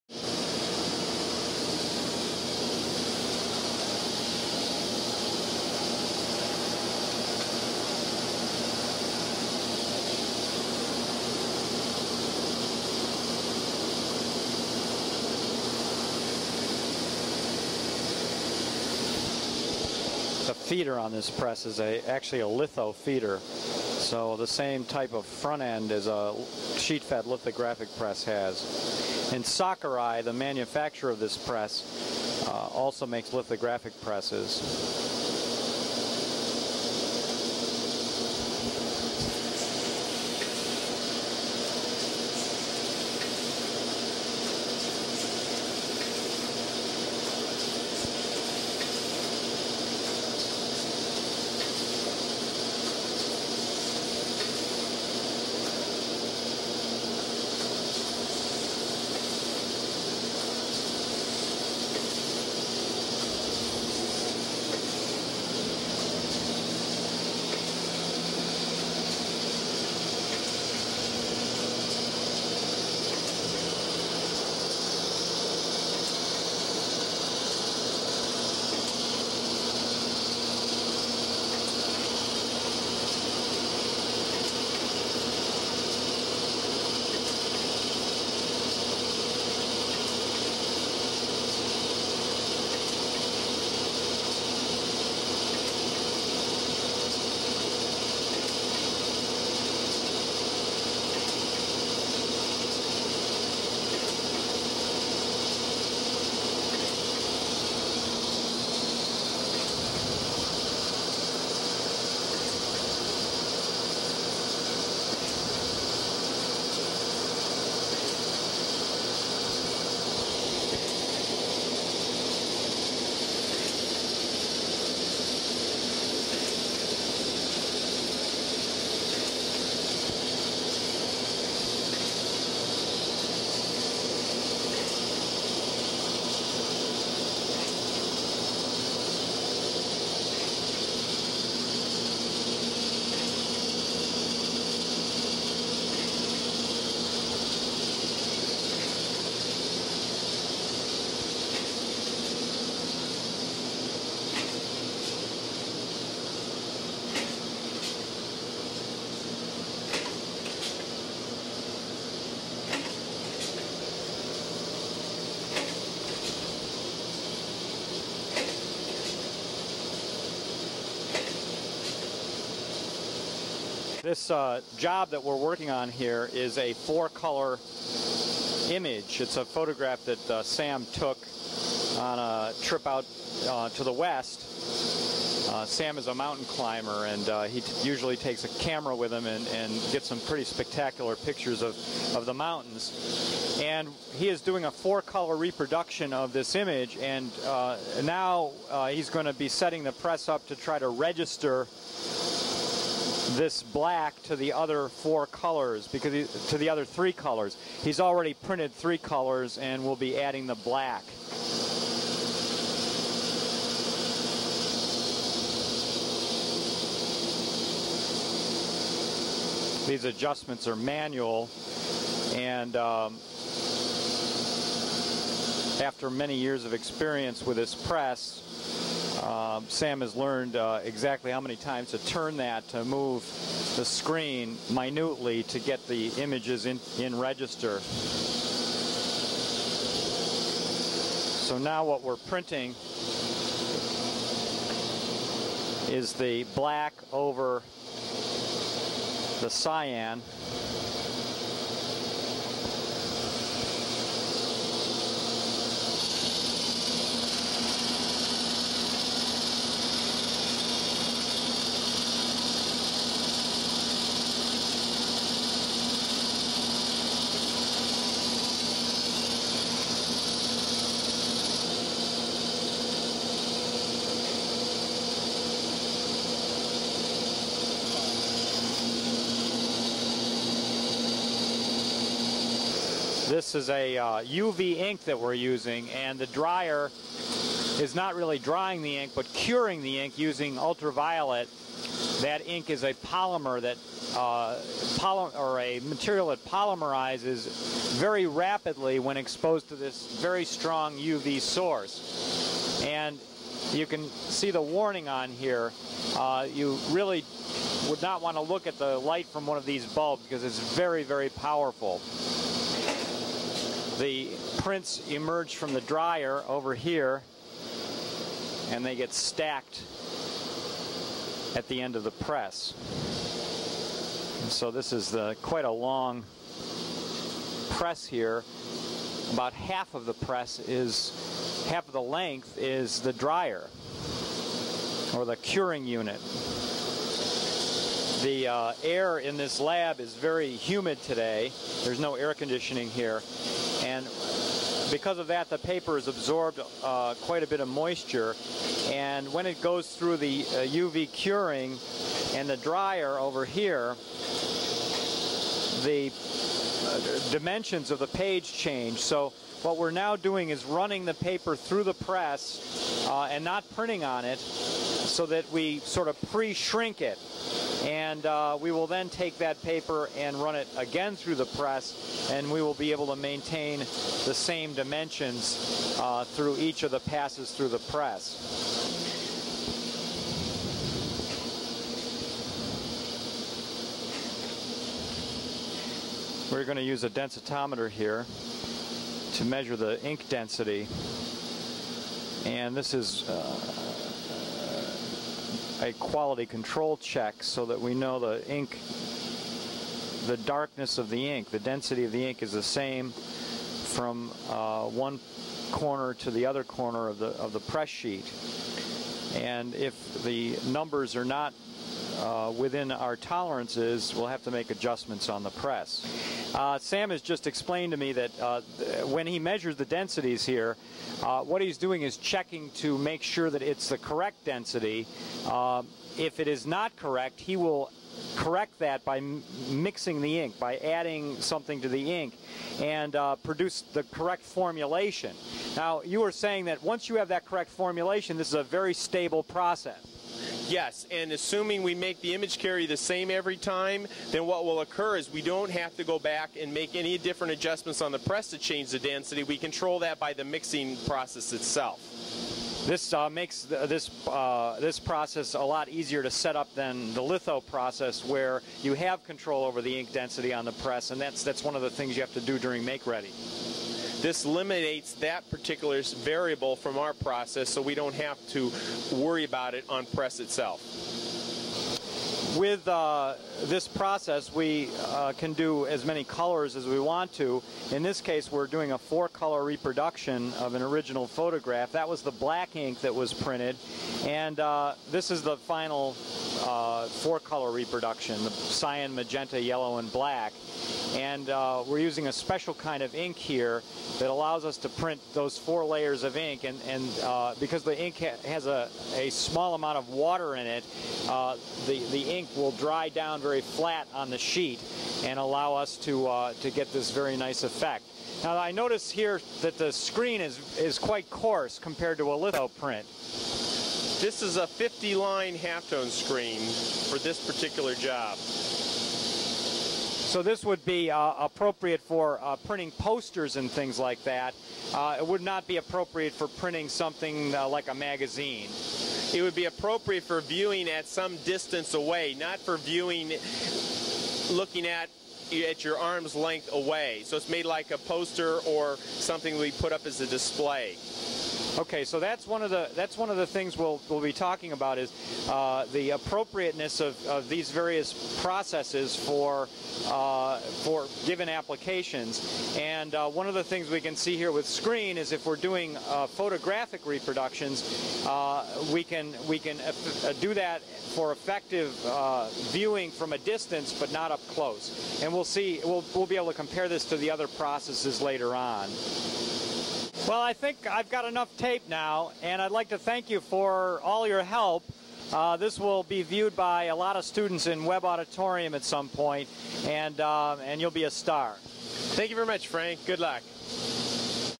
Sakurai Rotary Screen Printing Demonstration
This video was made in the mid 1990s for an introductory course in graphic communications at Rochester Institute of Technology. The video features Professor Sam Hoff.